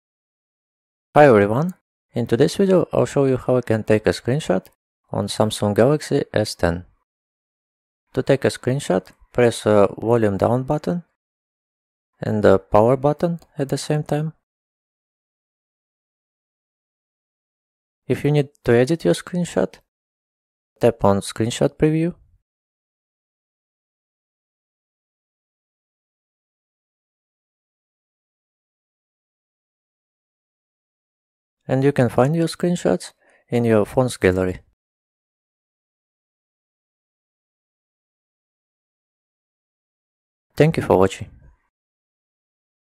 How to Take a Screenshot on Samsung Galaxy S10 Phone
In this video tutorial i will show you how to take a screenshot on Samsung Galaxy S10 phone!